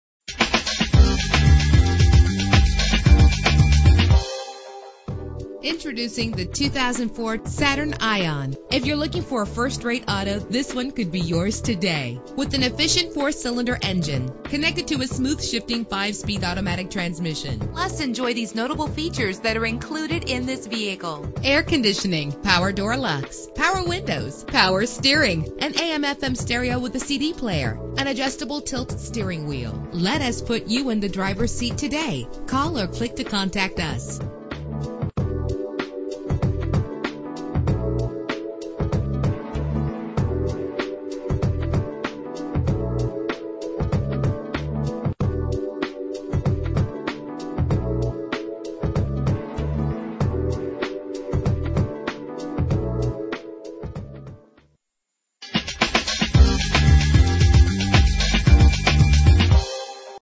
2004 Saturn ION - Rosenberg TX
Year: 2004
Make: Saturn
Model: ION
Trim: 2
Engine: 2.2 liter inline 4 cylinder 16 valve
Transmission: 5-Speed Automatic
Color: Green
Mileage: 108674
Address:
27309 SW Freeway